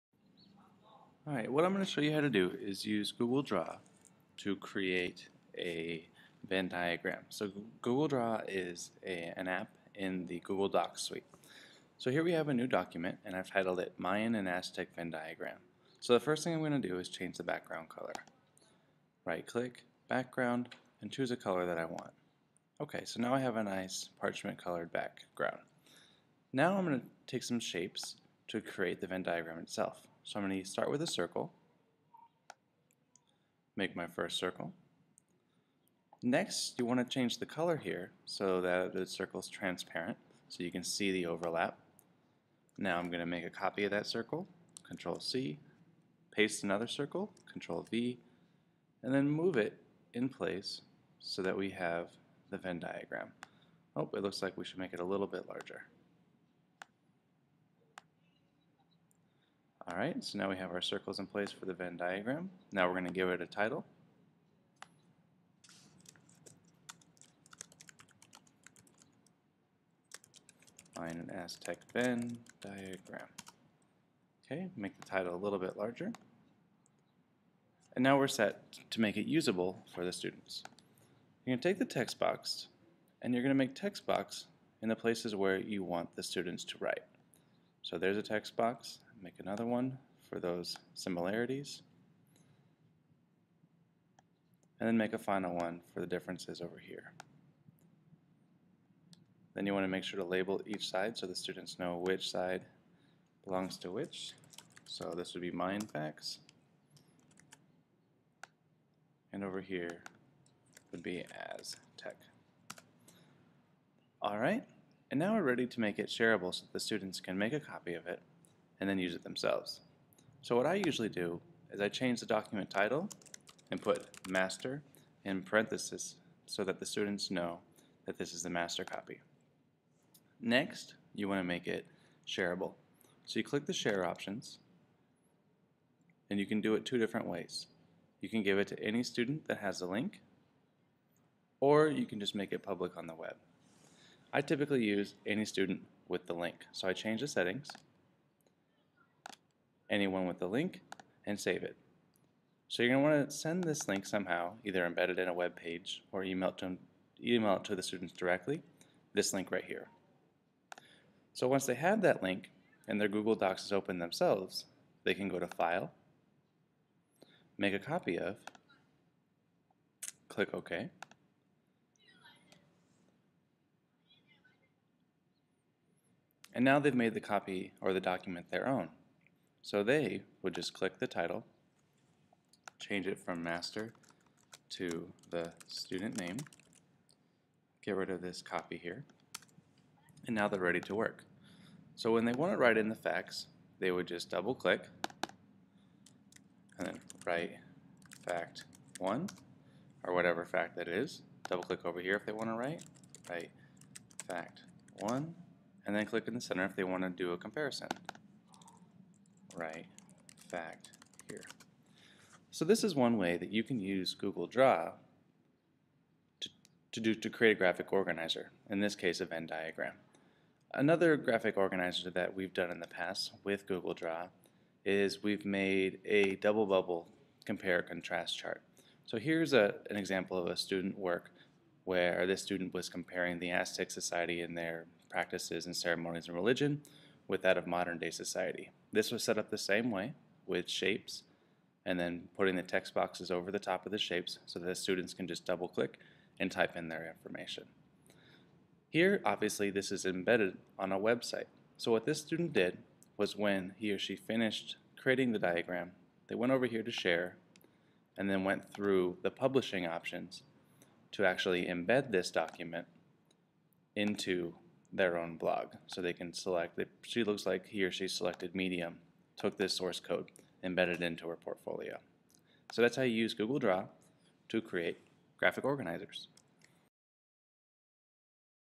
Using Google Draw to Create Graphic Organizers
Here is a video that demonstrates how to use Google Draw to create graphic organizers for your students to use in class.